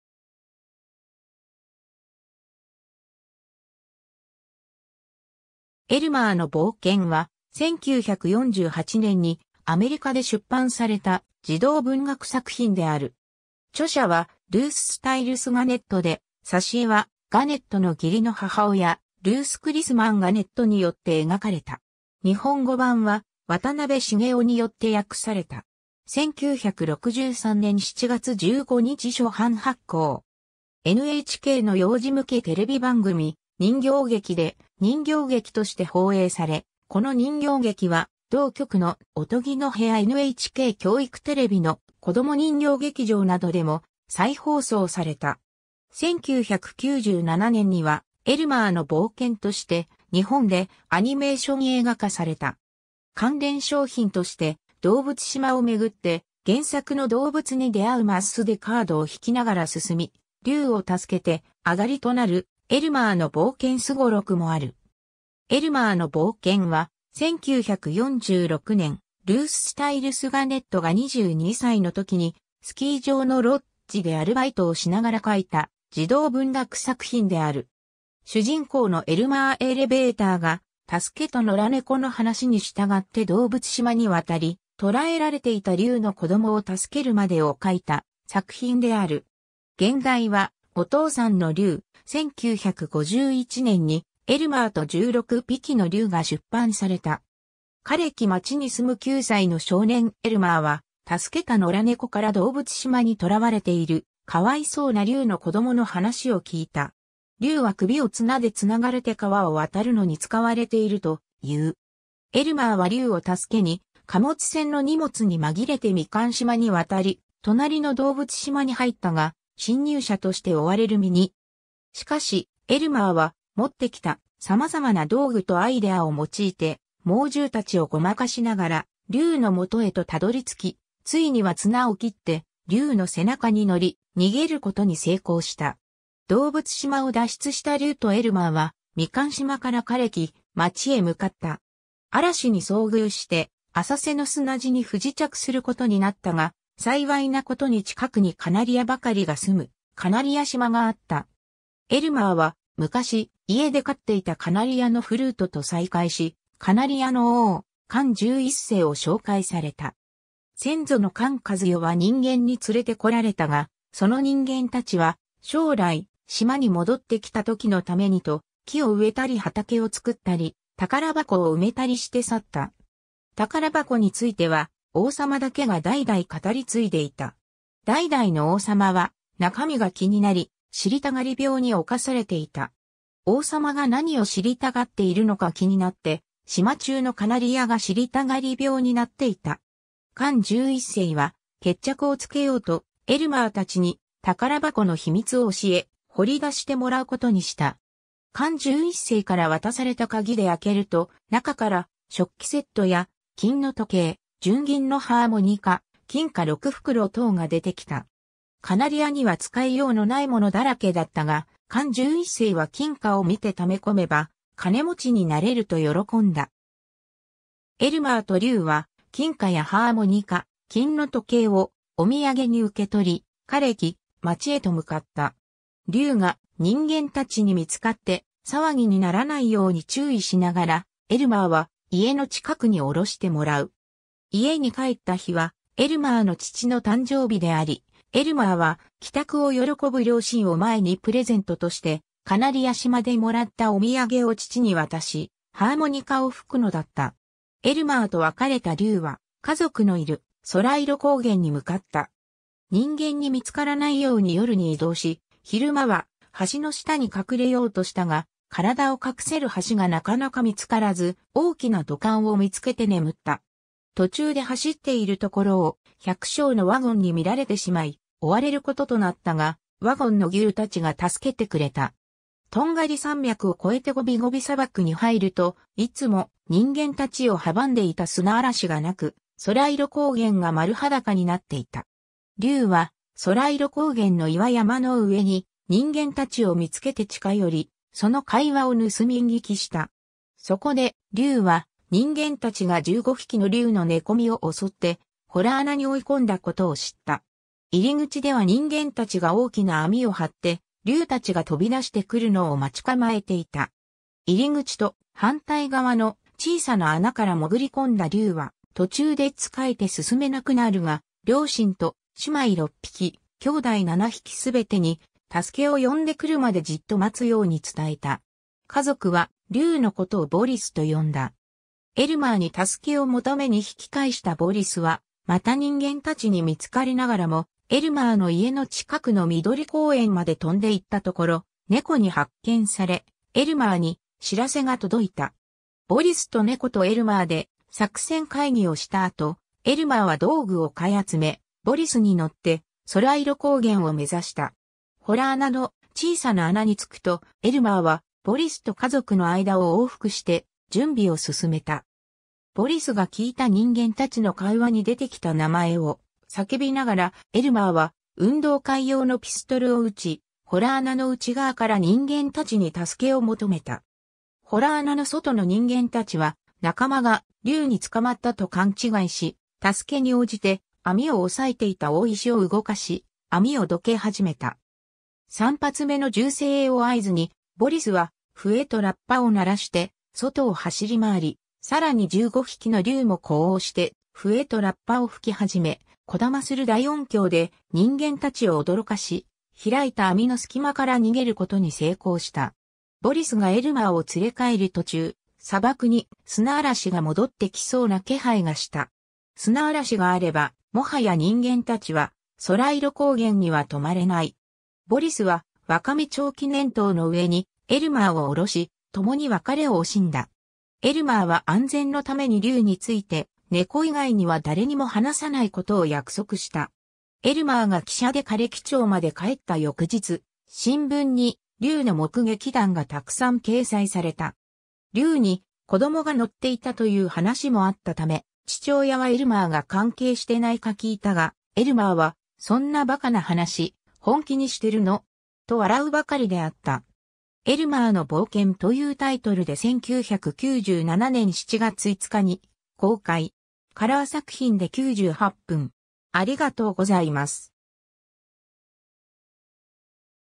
エルマーのぼうけん
『エルマーのぼうけん』（原題：My Father's Dragon）は、1948年にアメリカで出版された児童文学作品である。
著者はルース・スタイルス・ガネット (Ruth Stiles Gannett) で、挿絵はガネットの義理の母親ルース・クリスマン・ガネットによって描かれた。
日本語版は渡辺茂男によって訳された。
1963年7月15日初版発行。
 NHKの幼児向けテレビ番組『にんぎょうげき』で人形劇として放映され、この人形劇は同局の『おとぎのへや』NHK教育テレビの『こどもにんぎょう劇場』などでも再放送された。
1997年には、『エルマーの冒険』として日本でアニメーション映画化された。
 関連商品として、どうぶつ島を巡って原作の動物に出会うマスでカードを引きながら進み、りゅうを助けてあがりとなる「エルマーのぼうけんすごろく」もある。
 『エルマーのぼうけん』は、1946年、ルース・スタイルス・ガネット (Ruth Stiles Gannett) が 22歳の時にスキー場のロッジでアルバイトをしながら書いた児童文学作品である。
 主人公のエルマー・エレベーターが、助けた野良猫の話に従ってどうぶつ島に渡り、捕らえられていた竜の子供を助けるまでを書いた作品である。
 原題は 「お父さんの竜」（My Father's Dragon)で、語り手の父親(my father)が 9歳の少年だった時の話として書かれている。会話などで直接的に名前を使う時を除いて、ほとんどすべてが、主人公を「エルマー」ではなく 「my father」 と表現して語られていくが、2巻目である『エルマーとりゅう』からは「エルマー」を主語に用いている。日本語訳はシリーズ3作品が揃った後に出版され、 1作目の主語も「エルマー」としている。 両親の勧めで、ガネットは出版社のランダムハウスに持ち込んだ。とりあえず子供たちの反応を見る事にした編集者が、ある小学校の生徒たちに『エルマーのぼうけん』を読ませると大好評だった。しかし、担任の教員の報告書には「著者は少し頭がいかれてるのでは？」と書かれていた。 挿絵画家選びは難航したが、1946年に出版され、1947年にニューベリー賞最優秀賞(Newbery Winner)受賞作であるキャロライン・シャーウィン ベイリー (Carolyn Sherwin Bailey) の児童文学作品『ミス・ヒッコリーと森のなかまたち』(Miss Hickory)の挿絵を描いたルース・クリスマン・ガネットに頼むことになった。 ルース・クリスマン・ガネットはルース・スタイルス・ガネットの義理の母親でもある。 『エルマーのぼうけん』は1948年に出版されるとすぐに、ニューヨーク・ヘラルド・トリビューン (New York Herald Tribune) 春の児童図書祭賞(Children's Spring Book Festival Award)を受賞し、1949年にはニューベリー賞優秀賞(Newbery Honor)の 1つに選ばれている。 エルマーが竜を助けて家に帰ろうとするところで終わっているため、その続きとして1950年に『エルマーとりゅう』（原題：Elmer and the Dragon）、1951年に『エルマーと16ぴきのりゅう』（原題：The Dragons of Blueland）が出版された。
 かれき町に住む9歳の少年エルマーは、助けた野良猫からどうぶつ島に囚われている可哀想な竜の子供の話を聞いた。
 竜は首を綱で繋がれて川を渡るのに使われているという。
 エルマーは竜を助けに貨物船の荷物に紛れてみかん島に渡り、隣のどうぶつ島に入ったが、侵入者として追われる身に。
 しかし、エルマーは持ってきた様々な道具とアイデアを用いて猛獣達をごまかしながら竜の元へと辿り着き、ついには綱を切って竜の背中に乗り、逃げる事に成功した。
 どうぶつ島を脱出した竜とエルマーは、みかん島からかれき町へ向かった。
 嵐に遭遇して浅瀬の砂地に不時着する事になったが、幸いなことに近くにカナリヤばかりが住むカナリヤ島があった。
 エルマーは、昔、家で飼っていたカナリヤのフルートと再会し、カナリヤの王、カン十一世を紹介された。
 先祖のカン一世は人間に連れ...